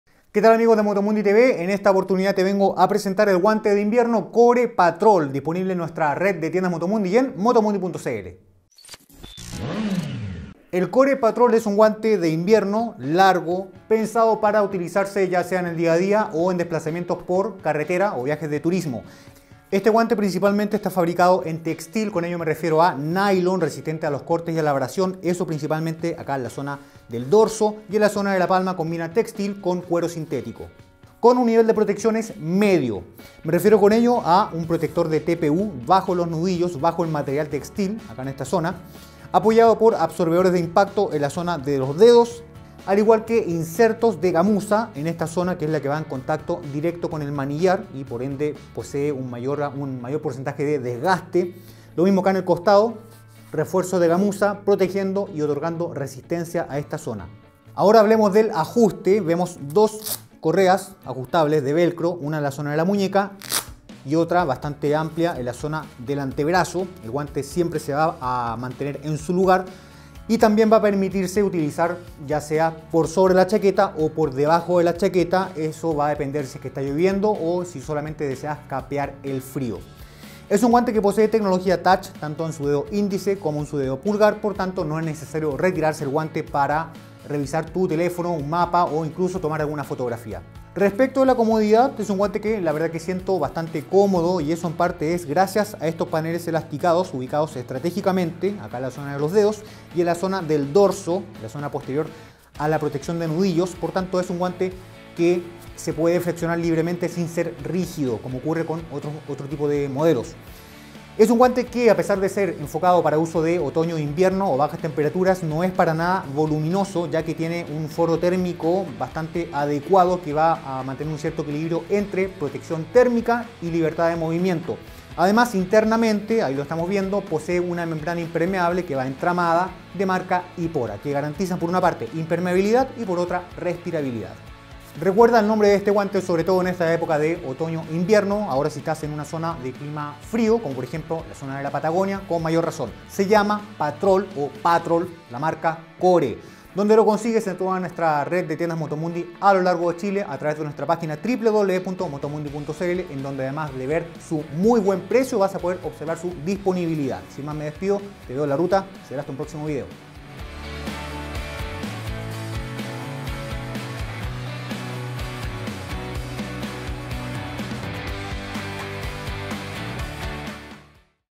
Guante Turismo - Invierno KORE PATROL disponibles en motomundi.cl
Guante turismo - invierno con protectores de TPU.
Confeccionado en poliéster y cuero de microfibra en palma.
Incluye membrana resistente al agua y forro térmico
Indice y pulgar con tecnología touch

Website Motomundi Chile Instagram Motomundi Chile Blog Motomundi Chile Facebook Tiendas